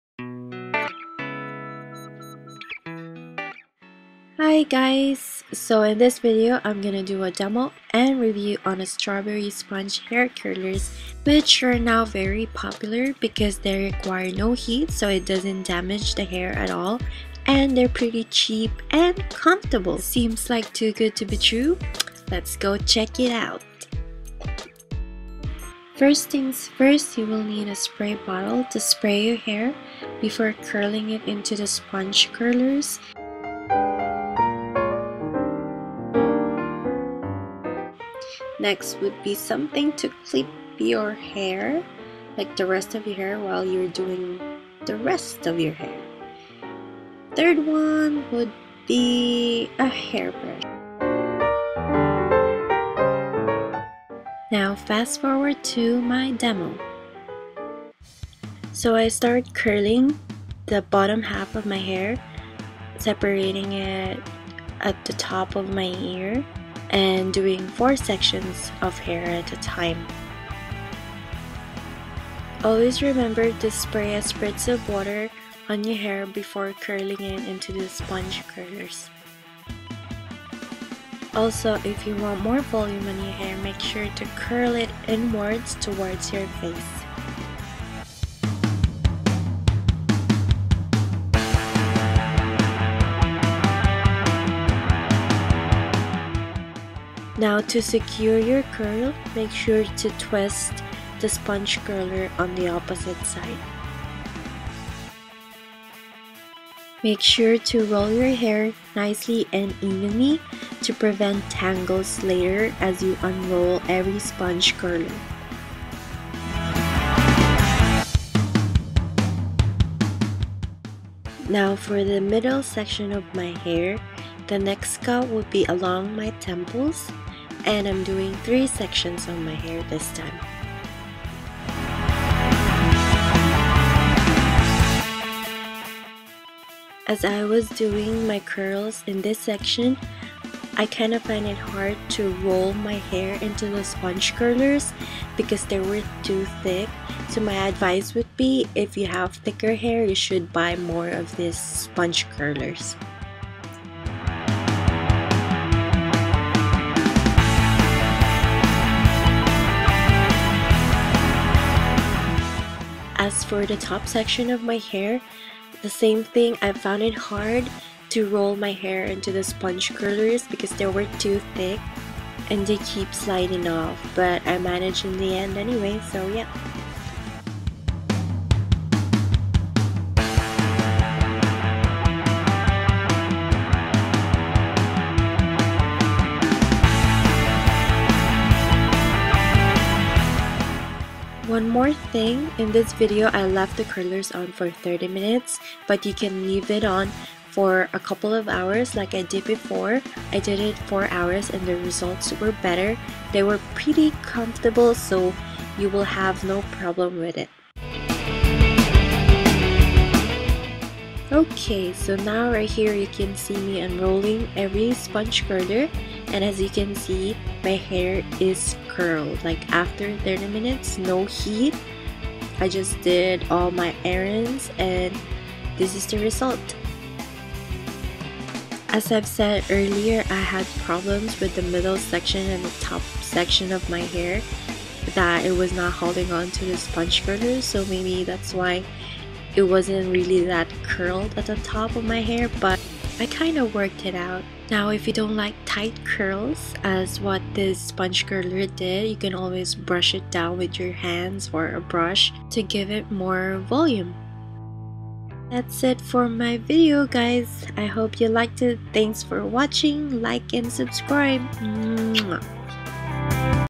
[DEMO/REVIEW] No Heat Strawberry Sponge Hair Curlers/Rollers! - aleleksm.
Demo and review on the strawberry sponge hair curlers!

Have a pretty day gorgeous! :*

*All editing resources are from iMovie.